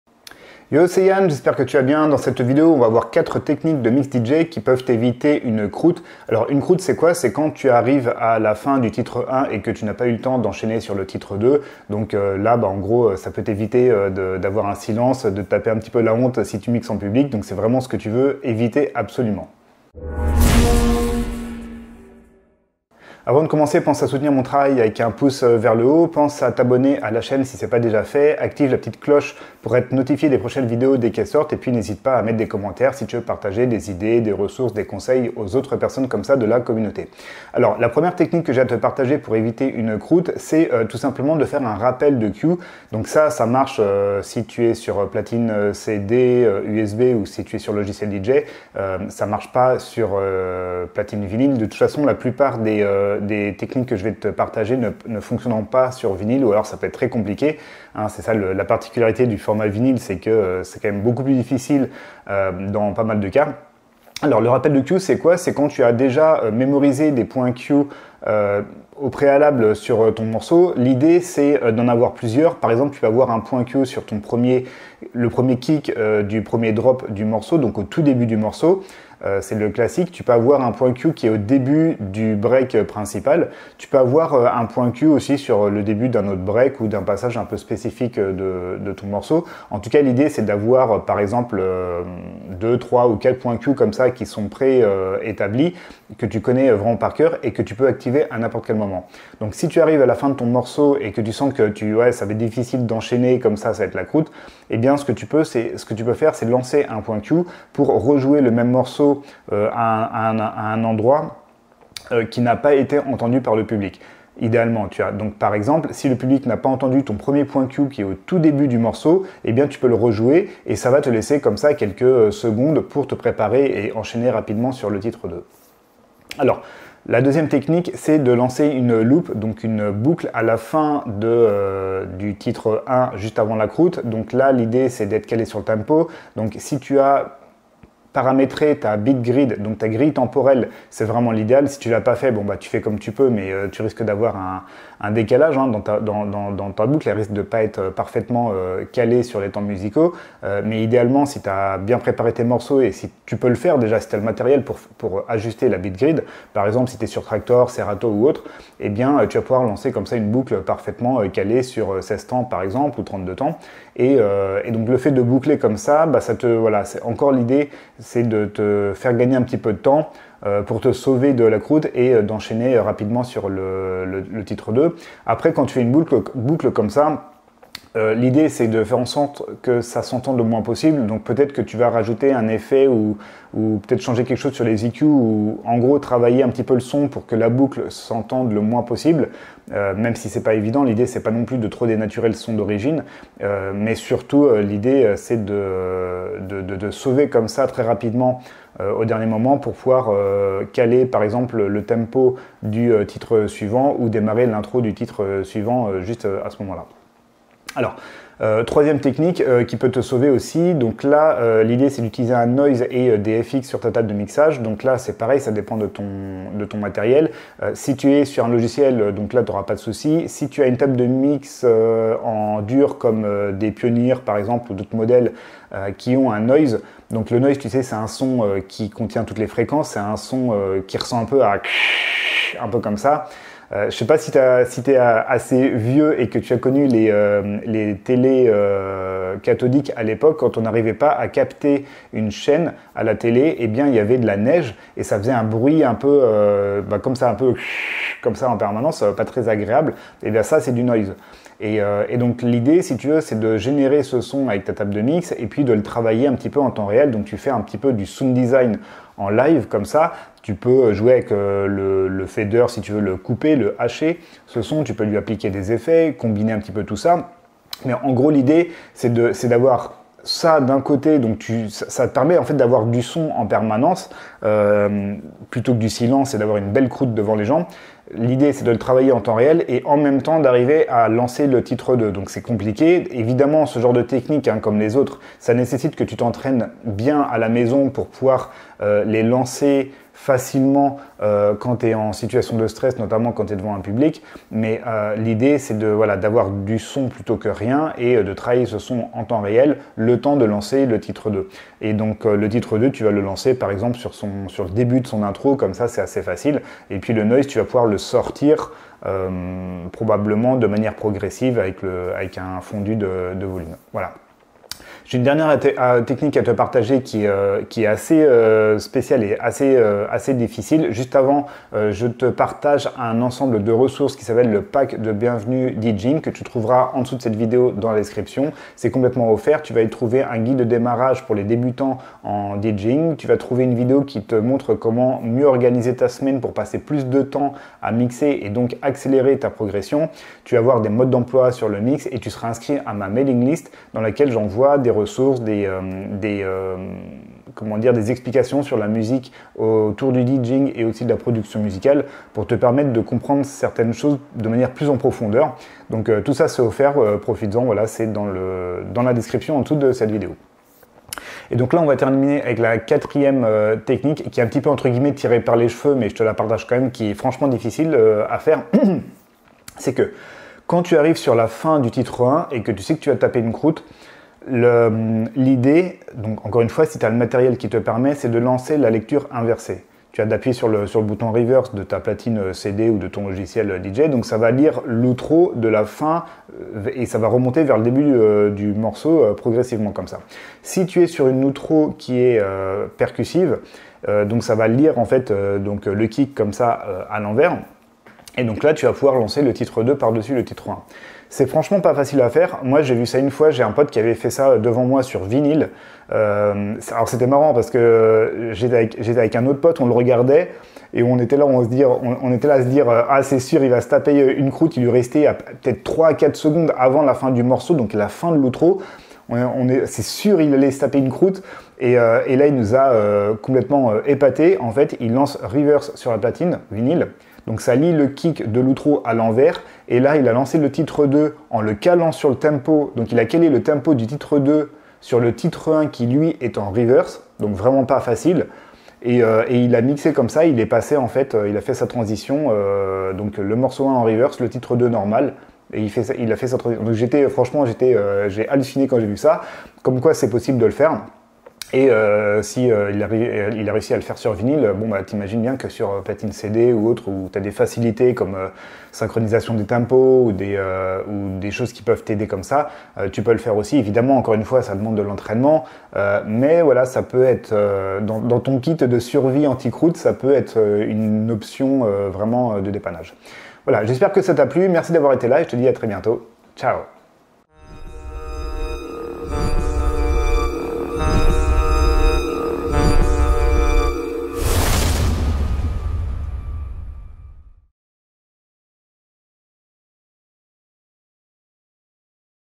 4 TECHNIQUES DE MIX DJ ANTI-CROÛTES
↓ ↓ ↓ ↓ PLUS D'INFOS CI-DESSOUS : ↓ ↓ ↓ ↓

Quand tu mixes, et surtout en public, ton cauchemar c'est de te taper une croûte, c'est-à-dire de ne pas avoir eu le temps d'enchaîner du titre 1 au titre 2 !

Résultat : un silence qui paraît durer une éternité, bref la loose !  :-/

Dans cette vidéo, je te partage 4 techniques de mix DJ pour éviter d'en arriver là, et qui peuvent te sauver la vie...

Yann

Par Yann Costaz (fondateur) & Damian Rocks (formateur DJing).